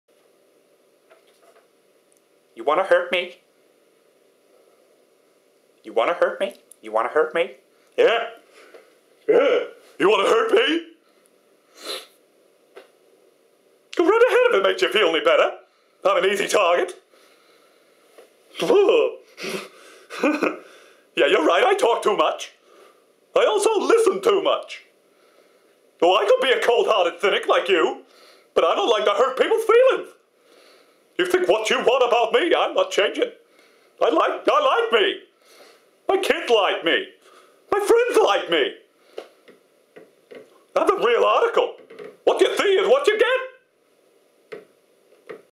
Mad Hatter impression? - You wanna hurt me?: out-take
Whilst trying to remember the words for the "You wanna hurt me?" monologue from planes, trains and automobiles/Family Guy, I kinda turned into Ed Wynn/the mad hatter for some reason...thought you might like it :)